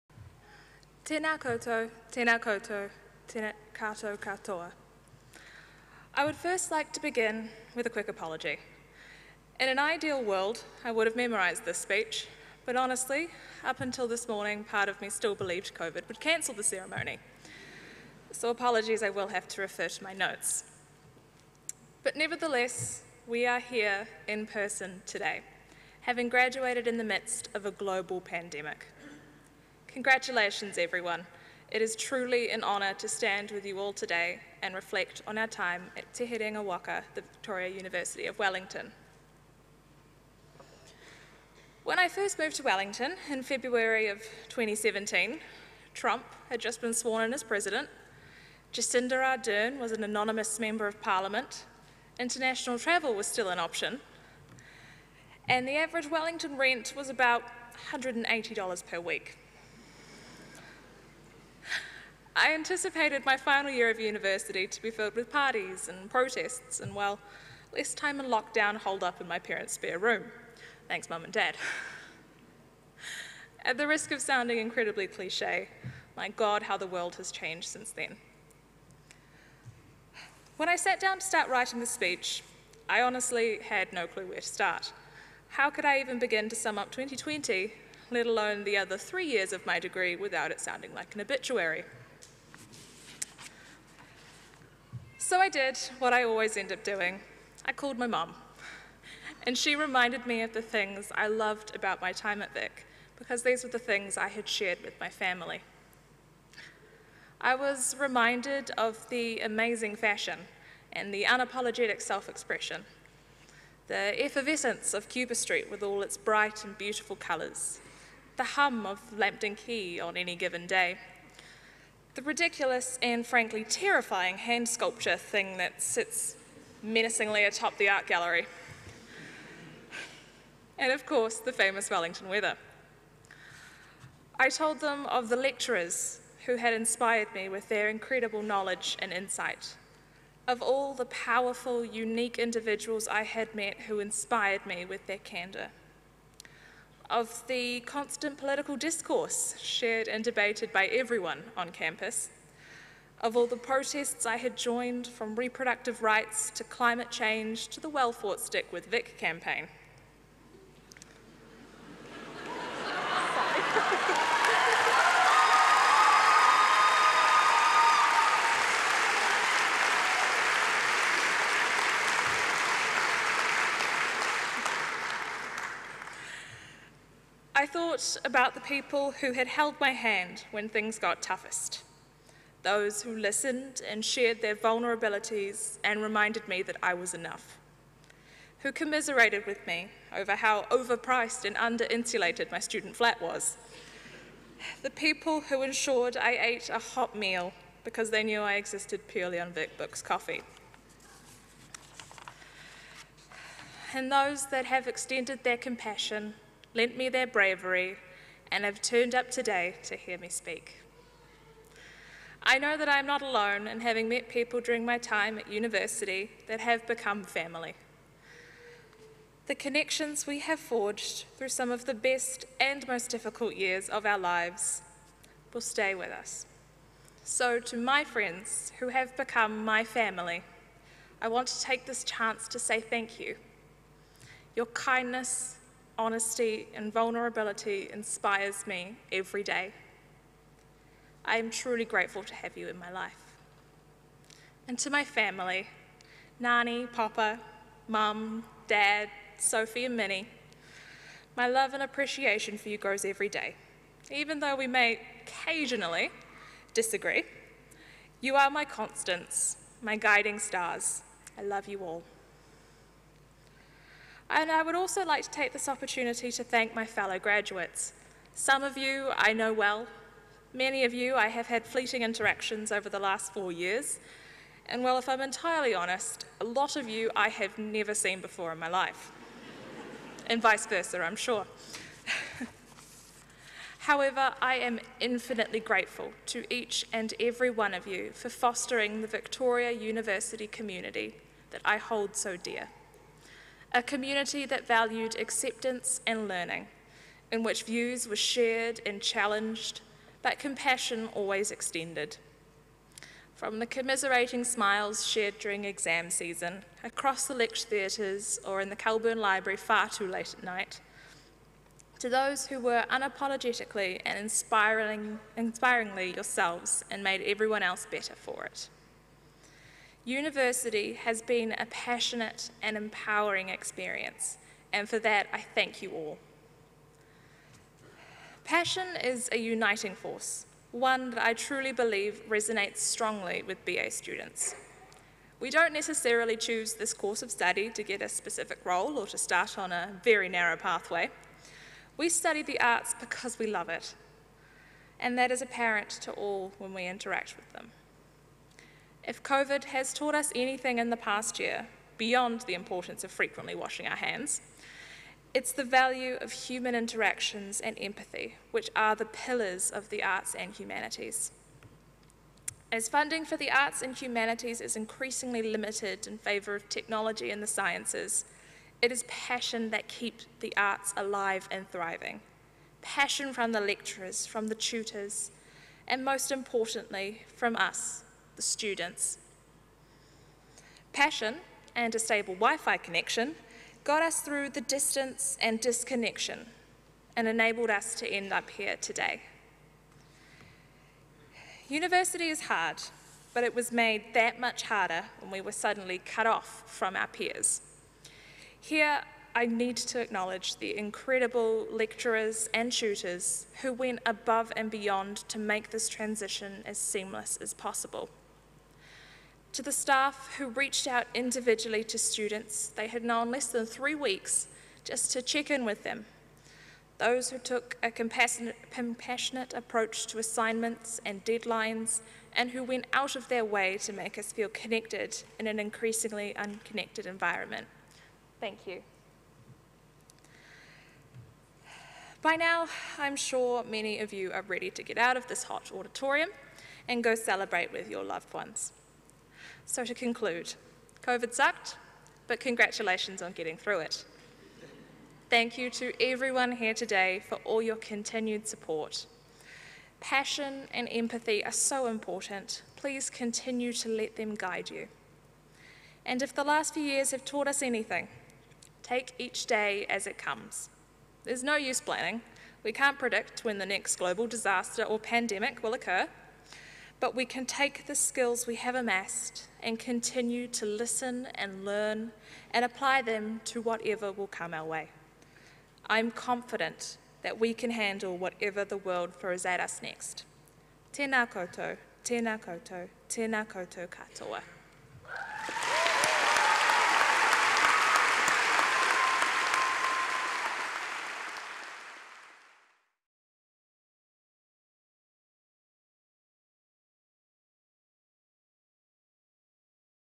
Graduate Speaker C5, May 2021 - Charlotte Doocey
Charlotte Doocey gave the graduate speech at Ceremony 5 on 20 May 2021